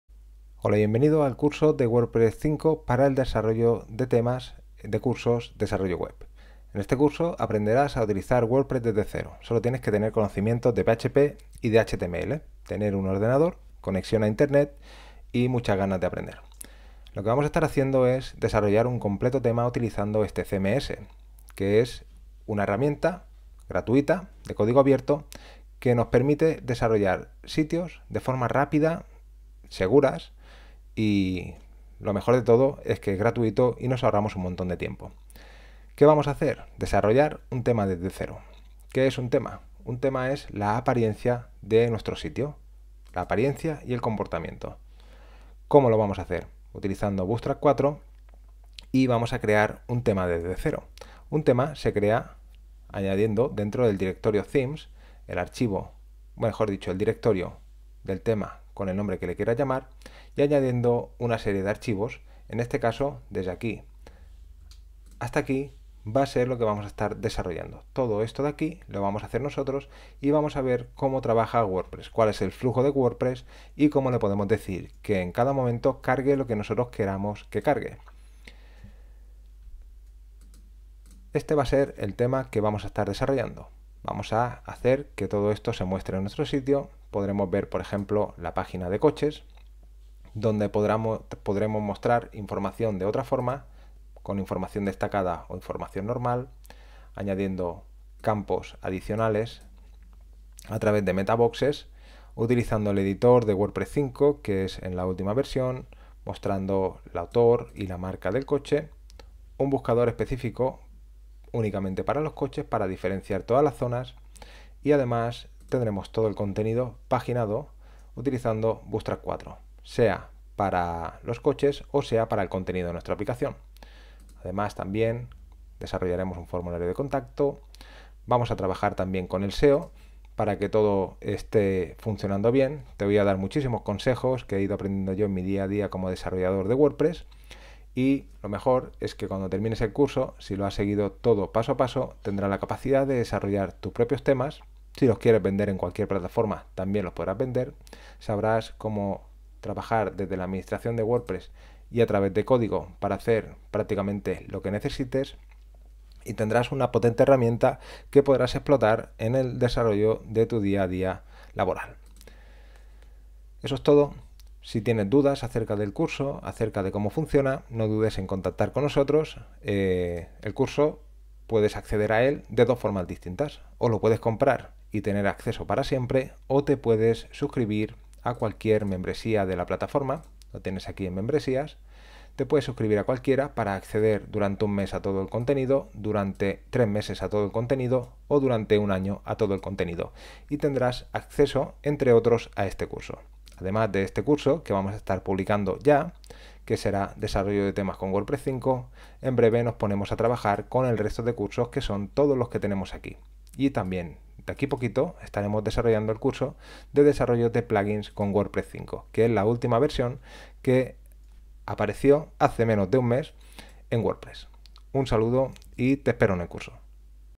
Desarrollo de temas con WordPress 5
El curso de WordPress 5 te ofrecerá todo lo necesario para que aprendas a programar con el CMS más popular y así acelerar tus desarrollos web.